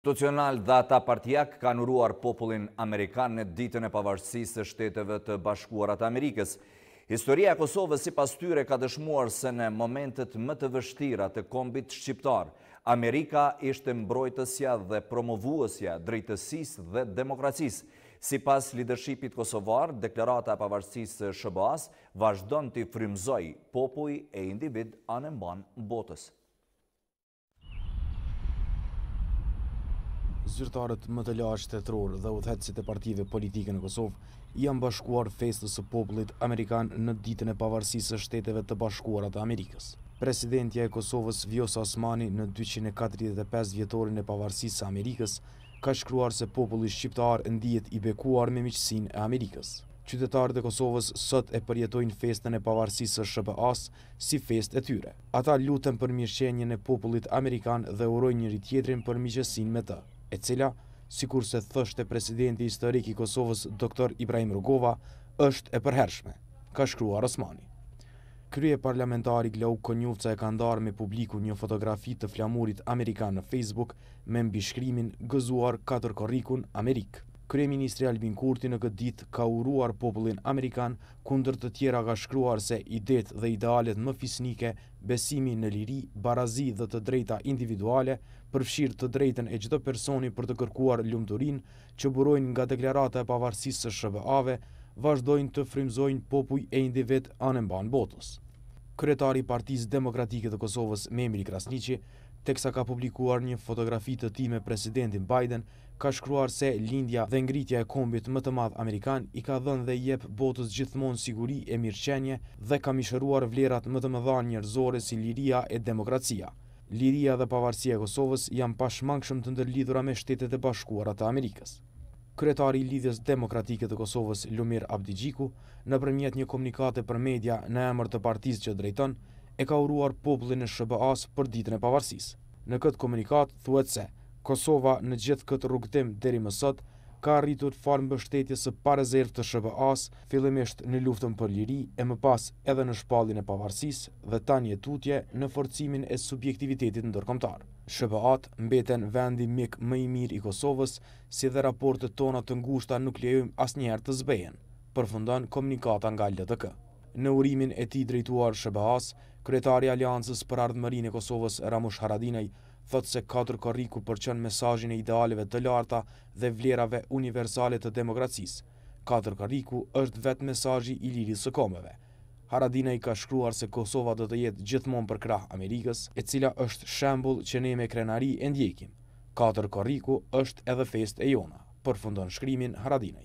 Osmani: Kosovarët e përjetojnë festën e pavarësisë së SHBA-së si festë të tyre
Follow RTK:Osmani: Kosovarët e përjetojnë festën e pavarësisë së SHBA-së si festë të tyre